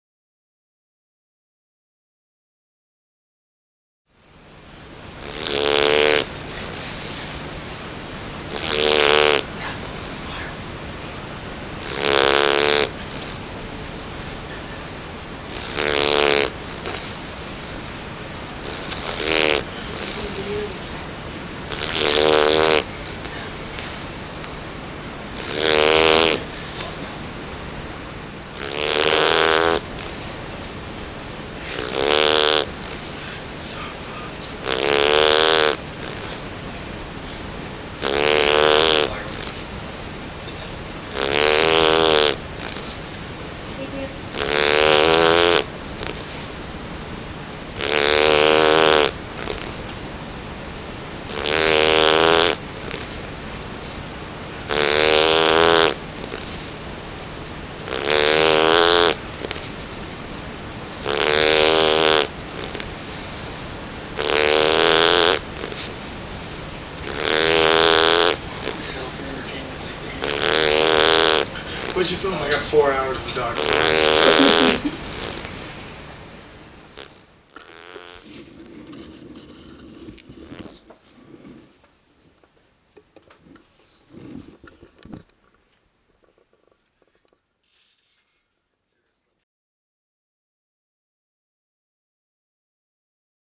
Adorable Puppy Snoring - Bernese Mountain Dog -
A quick Video of my Puppy Snoring. He's a Bernese Mountain Dog just over 9 weeks old.